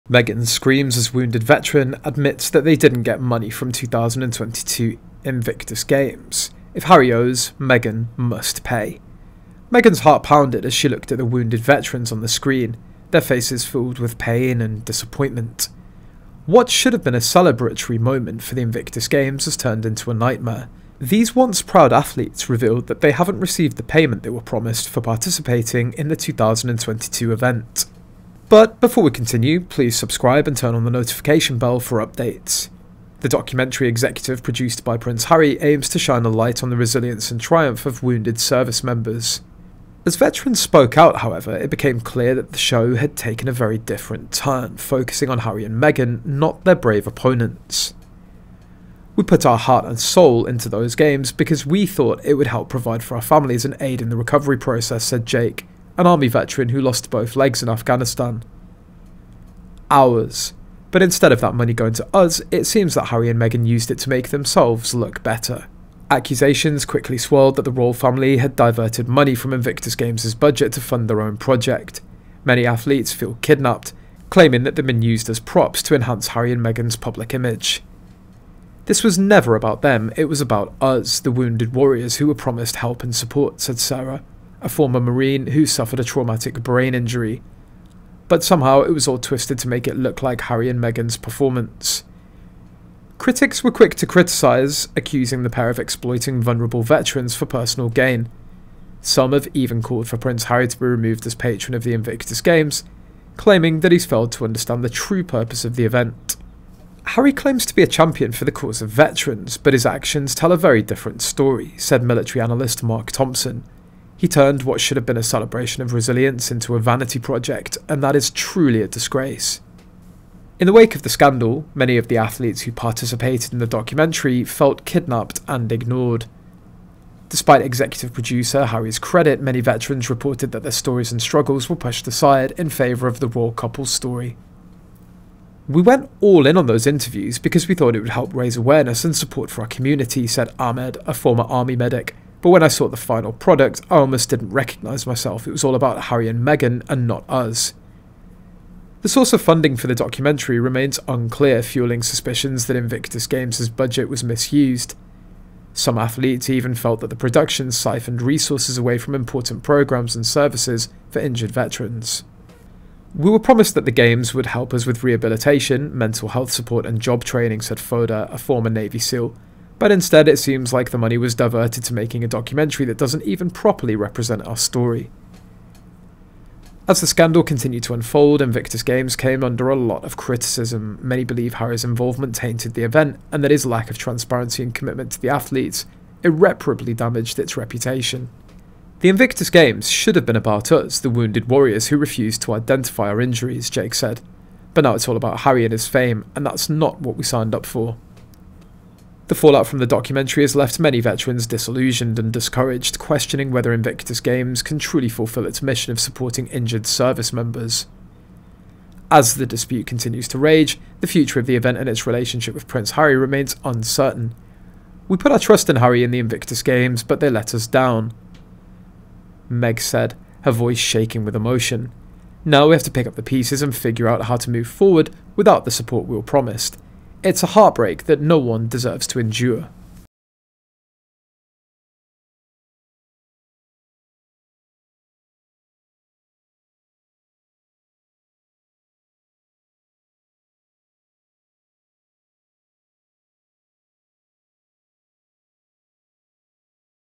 U HAVE TO PAY HAZ DEBT! Meg Boiled As Wounded Veteran Admit They Didn't Get Money From 2022 IG
Meg's heart pounded as she looked at the wounded veterans on the screen, their faces filled with pain and disappointment. What should have been a celebratory moment for the Invictus Games has turned into a nightmare. These once-proud athletes revealed that they have not received the payment they were promised for participating in the 2022 event.